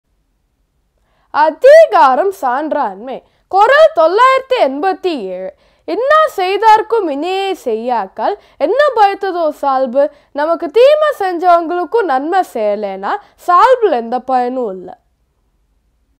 Kural 987 - Adikaram Sandranmai - Thirukkural with a simple meaning #987
Of what use is being noble
If one can’t do good unto those who did evil.